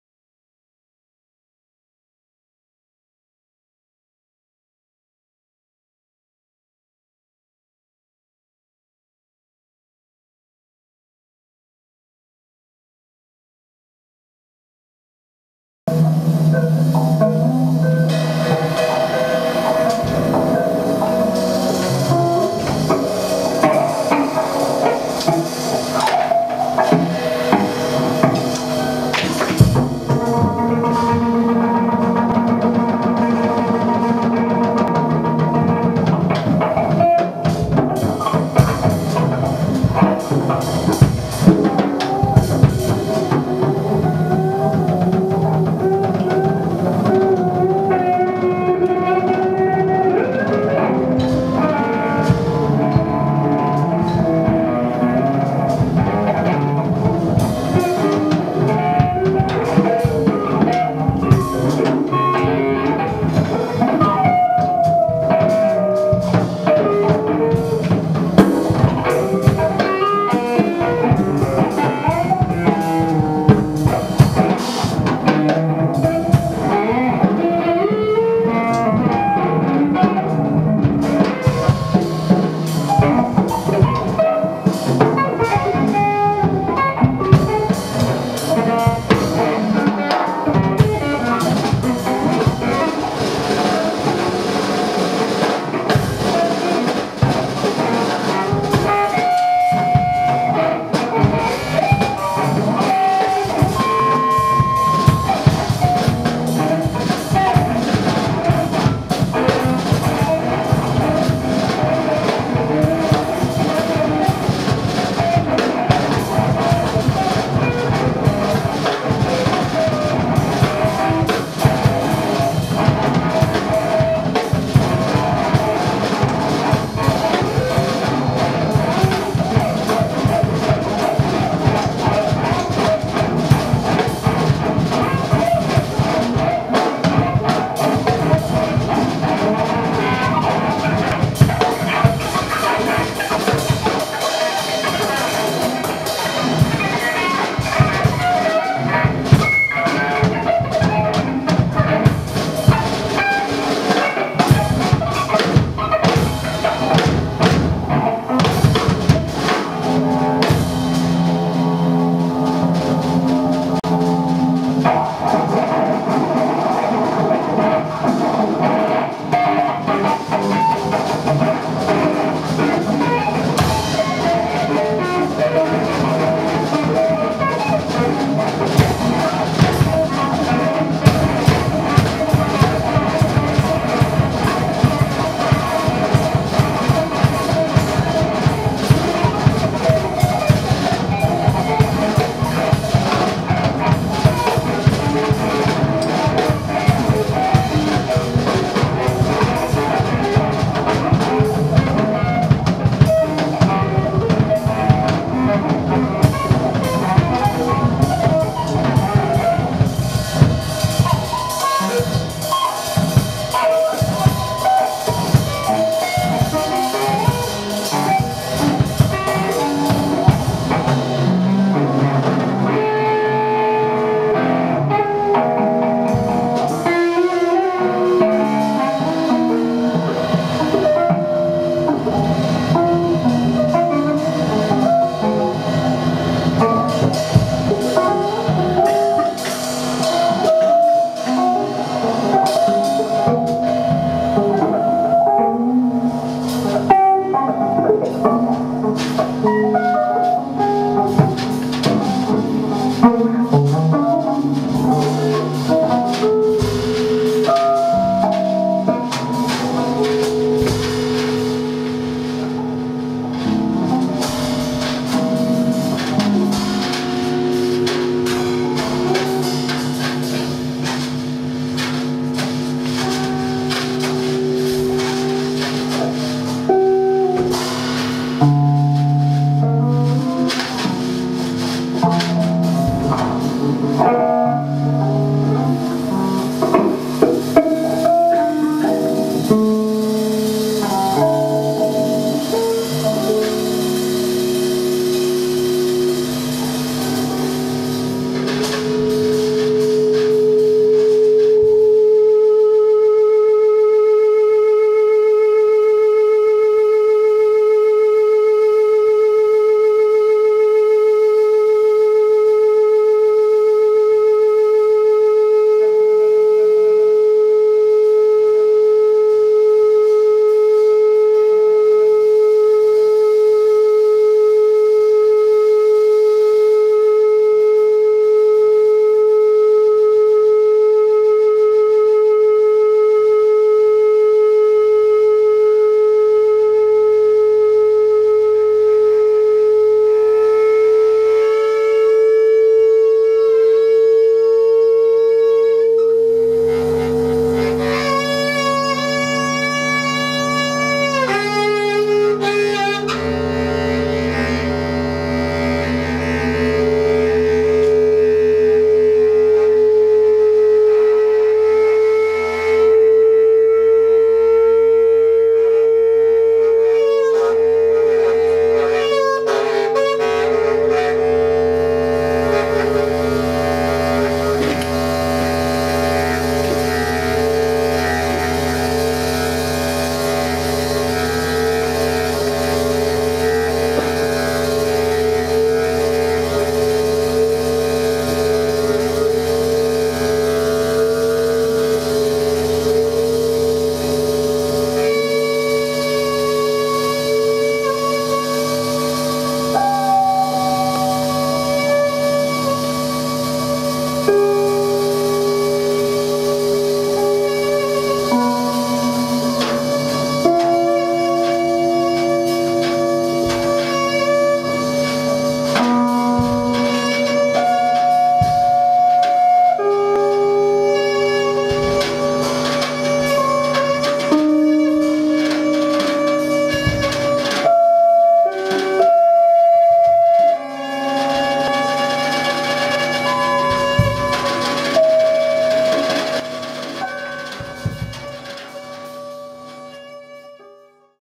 Chris Corsano + Manuel Mota - Live at ZDB Gallery
Chris Corsano: drums and percussion; Manuel Mota: electric guitar. Video recorded live by Nuno Moita at the ZDB Gallery in Lisbon on June 6, 2007.